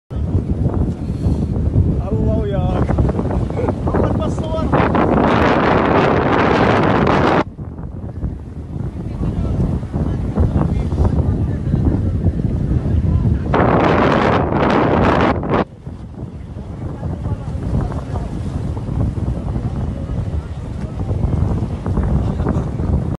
انهيار جسر سعد الرابط بين قضاءي شط العرب والدير شمالي البصرة بسبب المد البحري وارتفاع مناسيب المياه
انهيار جسر سعد الرابط بين قضاءي شط العرب والدير شمالي البصرة بسبب المد البحري وارتفاع مناسيب المياه فضلًا عن الإهمال الحكومي لصيانته
ـــــــــــــــــــــــ